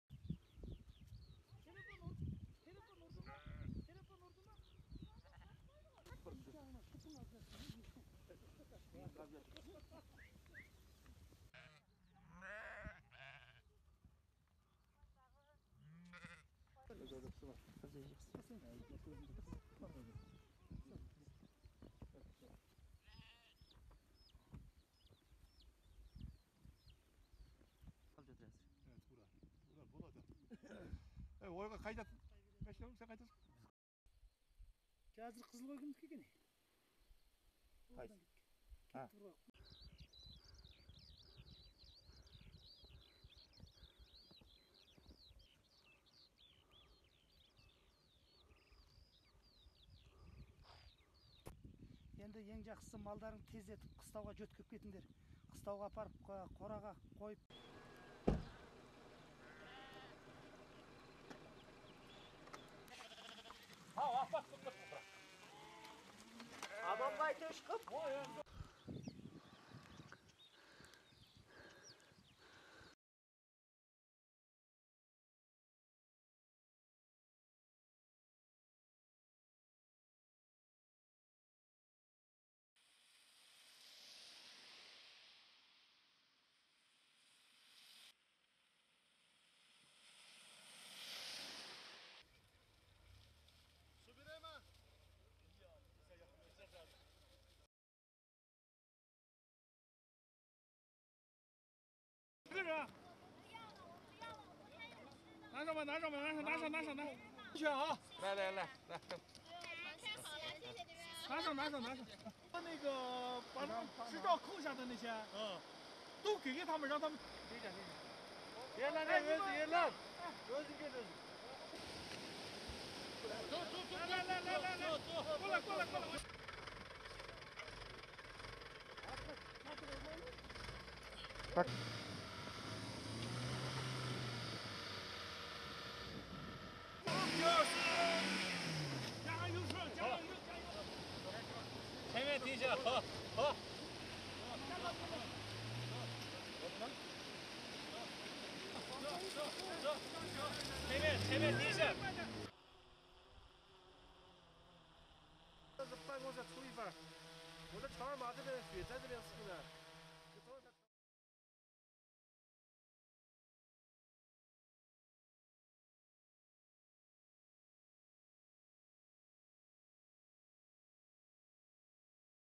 Blizzard Hit Xinjiang, Leading to Death of Livestock, Disturbing Traffic
Herdsmen in Tekes County, northwest China's Xinjiang Uygur Autonomous Region have endured heavy losses after Monday's blizzard froze to death over 400 livestock animals across the county.

The snow also disrupted traffic in Nilka County of Ili Kazak Autonomous Prefecture. Over 400 tourists in 130 vehicles were either stranded in the Tangbra Mountain or on the Dushanzi-Kuche Highway, which runs through the Tianshan Mountain from its south end to north end.